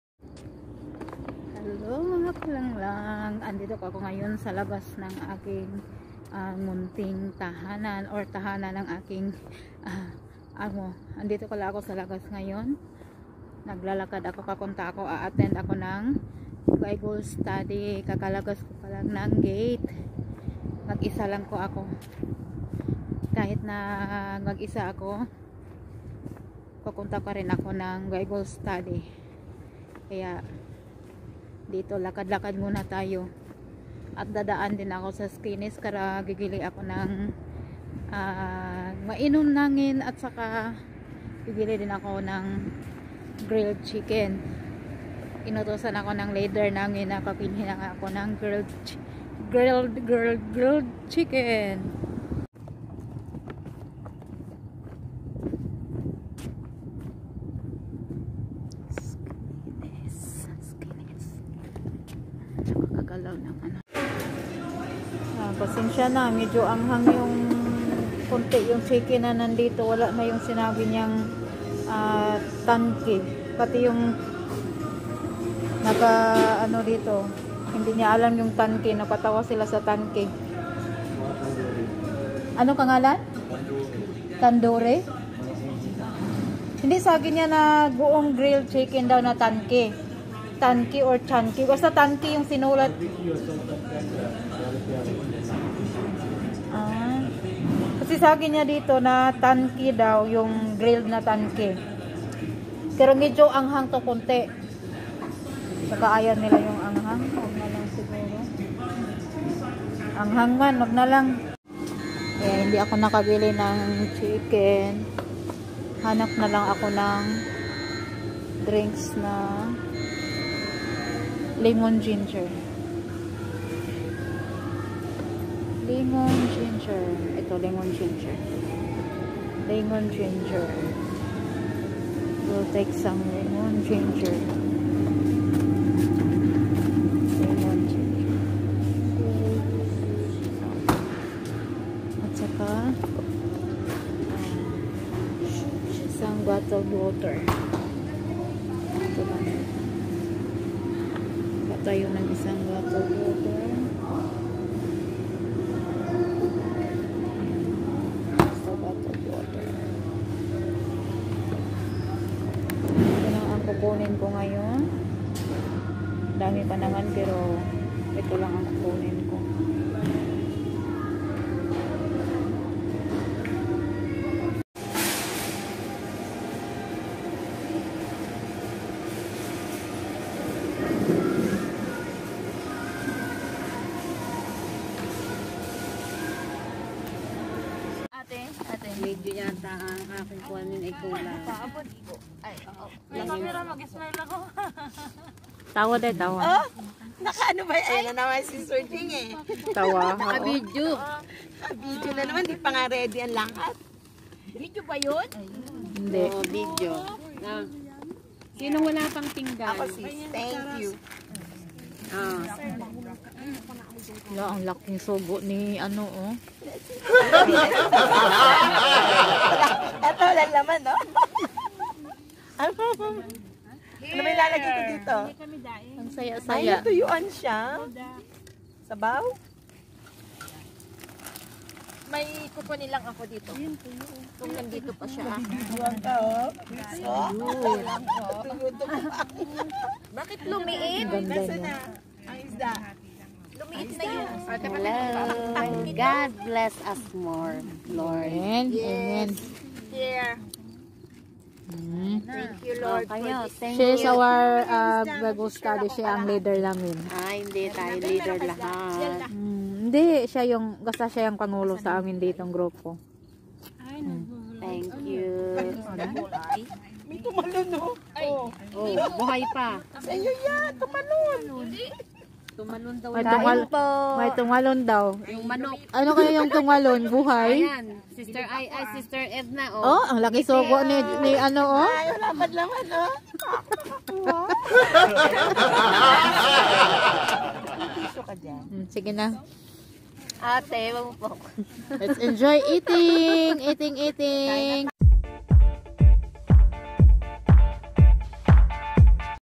Punta ng supermarket Fellowship dinner# Dubai ofw Gonzaga#
Sa nakasanayan tuwing araw ng Myerkules binigay ko na sa pag a attend ng bible study ang aking buhay. Malapit lang naman ang location ng meeting area namin. Ito ang mga ginawa namin tuwing Myerkules matapos mag sharing ng Word of God mag fellowship dinner na naman.